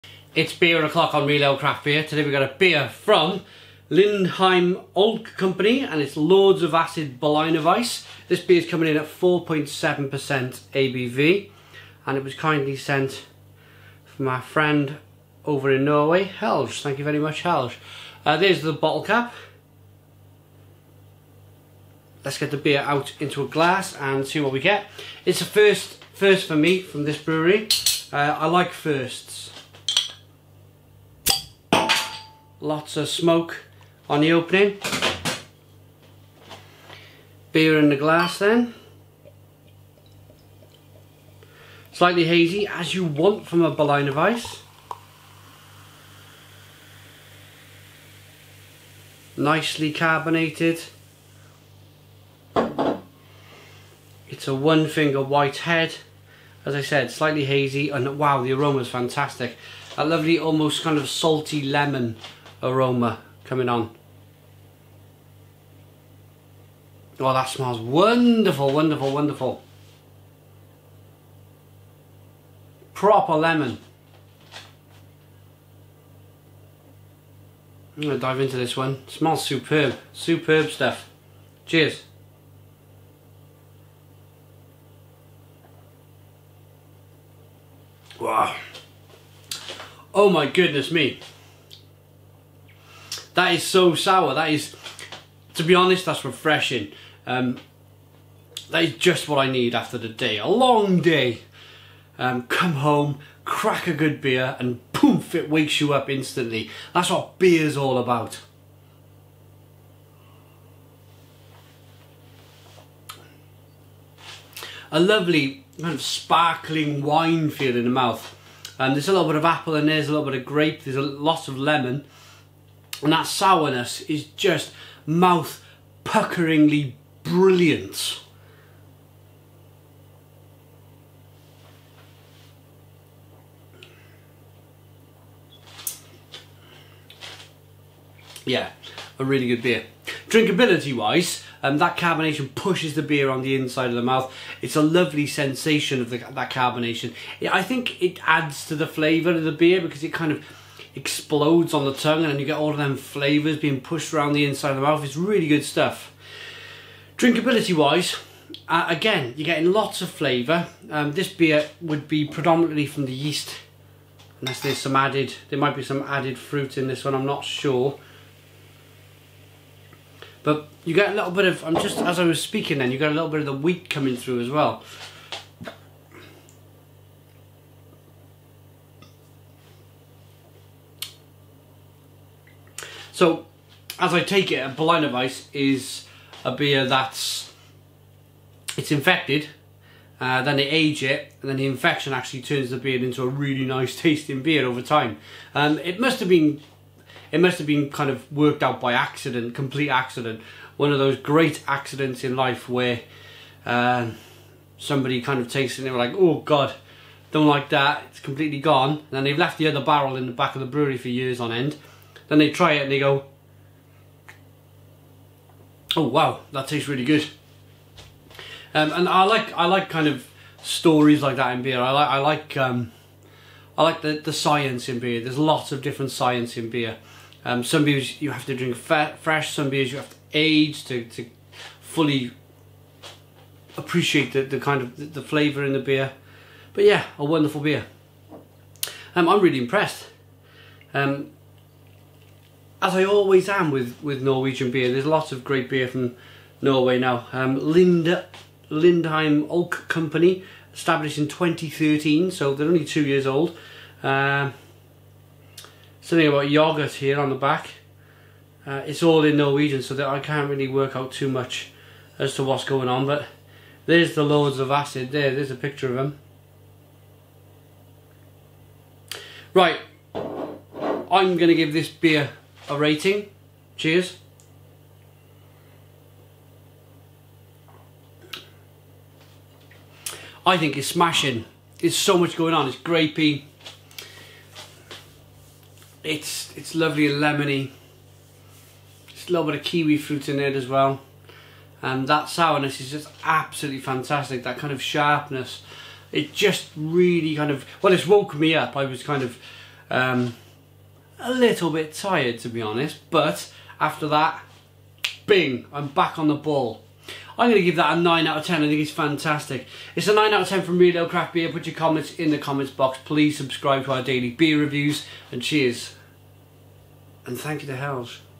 (4K) Lindheim Lords Of Acid Berliner Weisse By Lindheim Ølkompani | Norwegian Craft Beer Review
Recorded In 4K Ultra HD Real Ale Craft Beer Reviews Lindheim Lords Of Acid Berliner Weisse By Lindheim Ølkompani Lervig AktieBryggeri & Edge Brewing Company | Norwegian Craft Beer Review